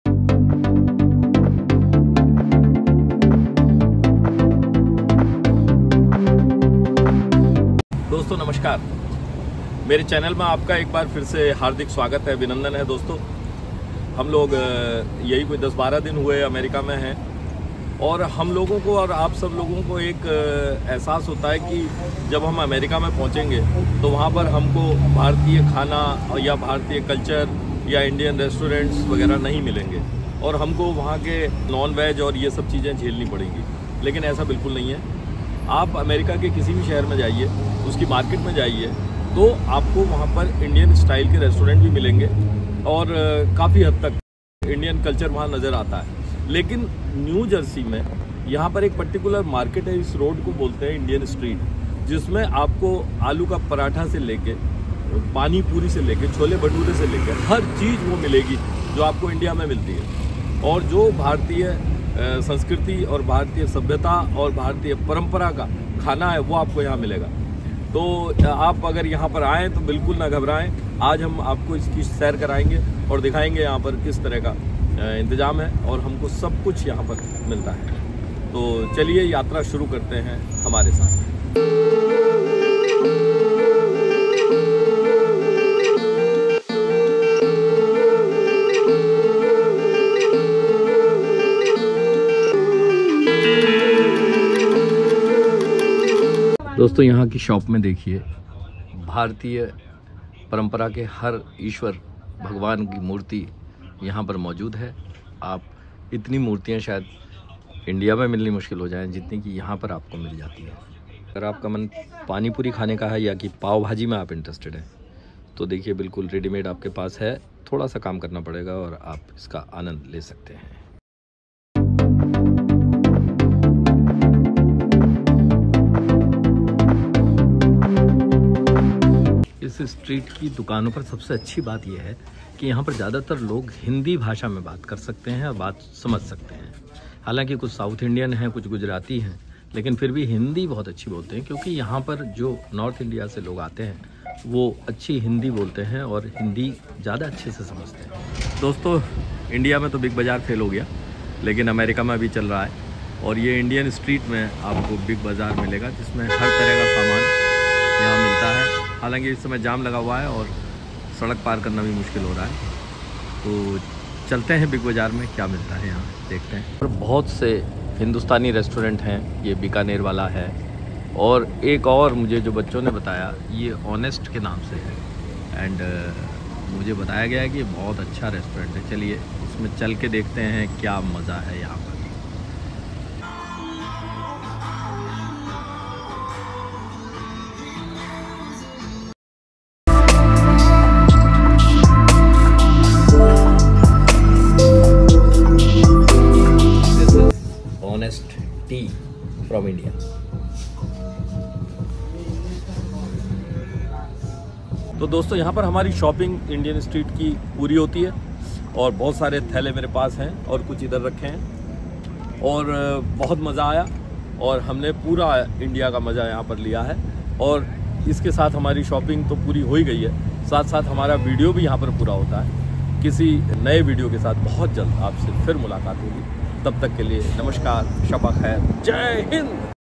WOW.....! "INDIAN STREET" IN THE USA HAS EVERYTHING..!
If you are in the USA and wish to enjoy Indian food items, raw marerial, spices or Indian dishes in true sense,  you can go to the Indian street in New Jersey which has everything as you can imagine in India. We have visited this street and have shown maximum possible things in this crispy and compact video. Hope you will enjoy it.